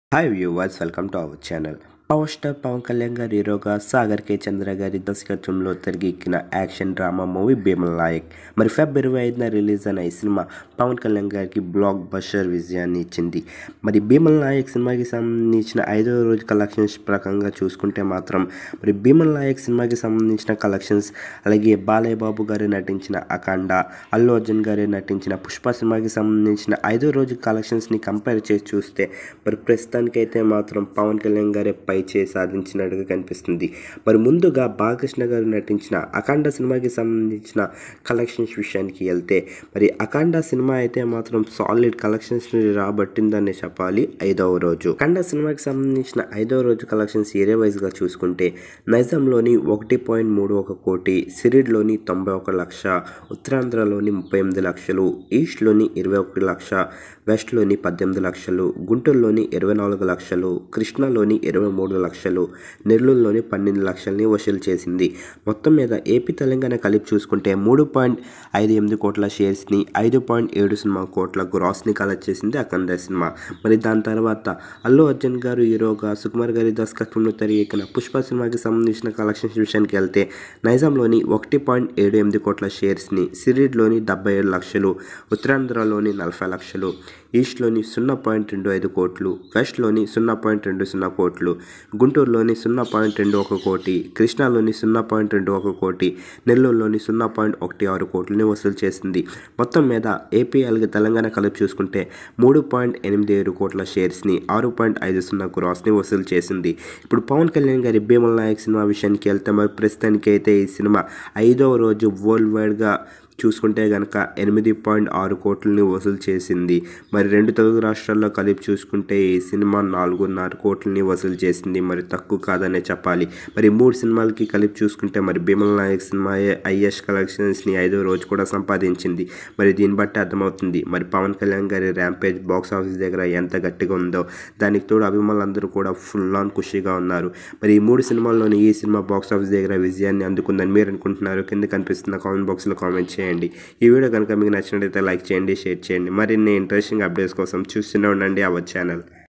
Bheemla Nayak Vs Akhanda Vs Pushpa Collections | Bheemla Nayak 5th day Total Collections |ManaPoster
Watch
Bheemla Nayak Vs Akhanda Vs Pushpa Collections | Bheemla Nayak 5th day Total Collections | Mana Poster